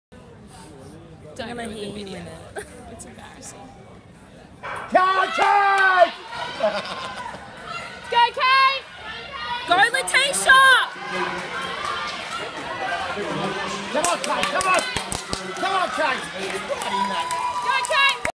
2017 Australian Championships - Kate U18 100mH Final
Women 100 Meter Hurdles U18
 Name Year Team Finals Alternate
Section 1 Finals Wind: -0.7
 1 3061 Willis, Lateisha 00 VIC 13.50
 2 2076 Newton-Smith, Camr 00 QLD 13.89
 3 790 Death, Tiana 00 NSW 13.91
 4 2687 Souter, Kajsa 00 QLD 14.13
 5 1297 Hickie, Jane 00 TAS 14.34
 6 2928 Vine, Doveinah 00 NSW 14.50
 7 1997 Morgan, Kathryn 00 VIC 14.79
 8 1301 Hicks, Caitlyn 00 NSW 14.91